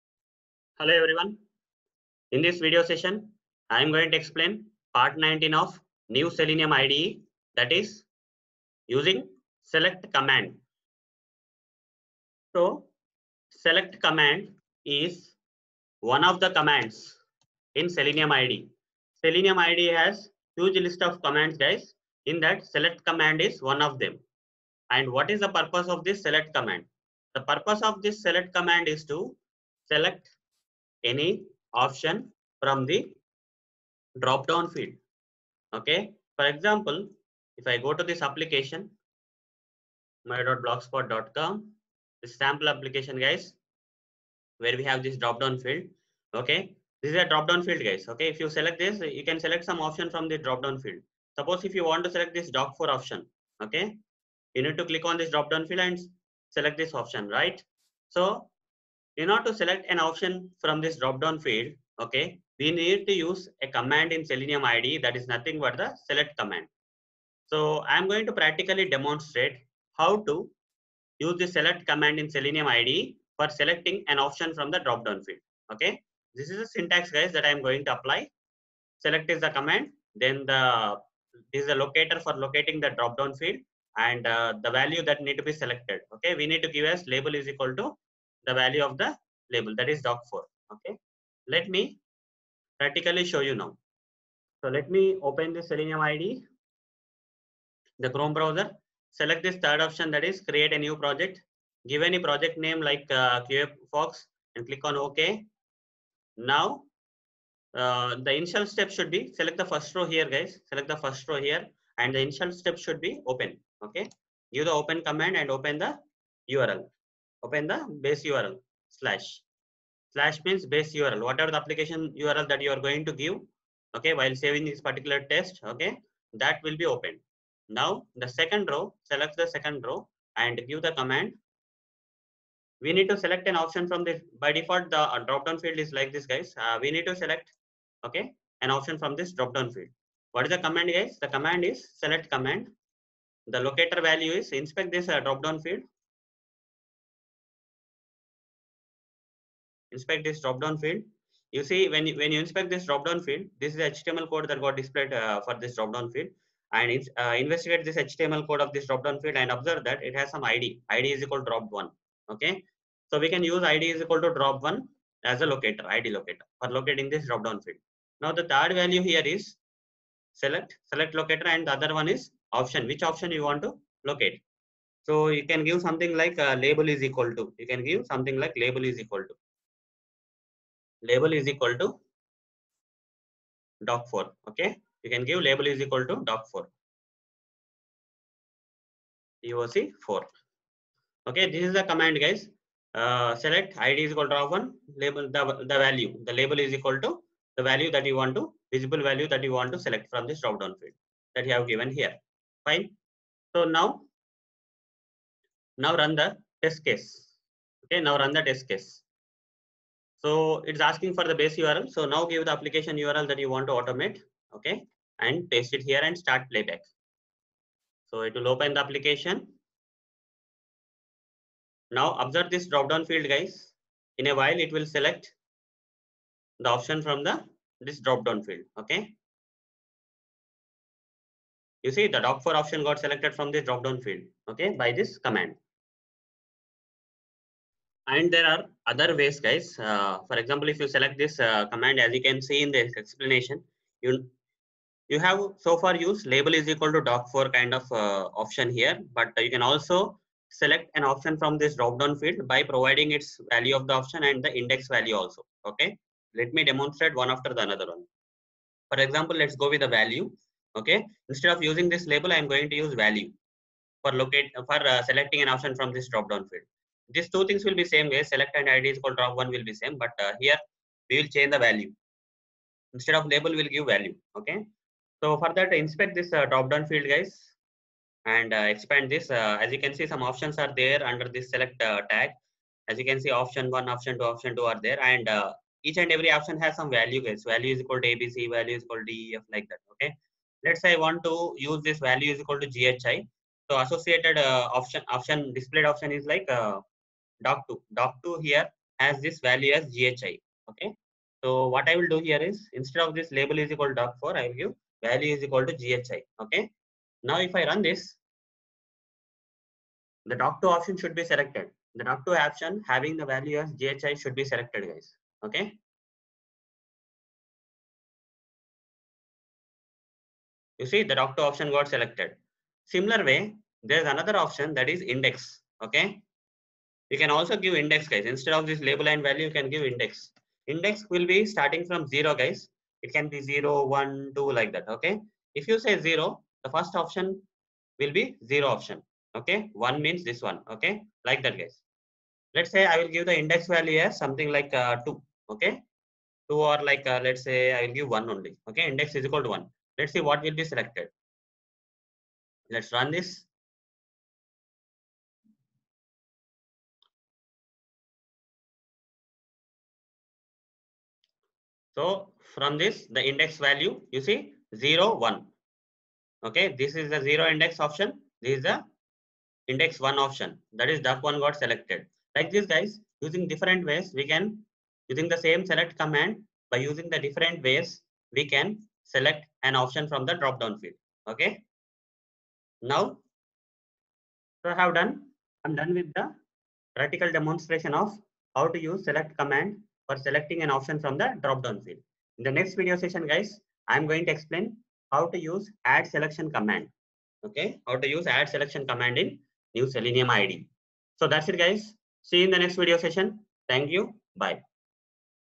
New Selenium IDE - Part 19 - select Command
In this video, I have practically demonstrated using 'select' command in New Selenium IDE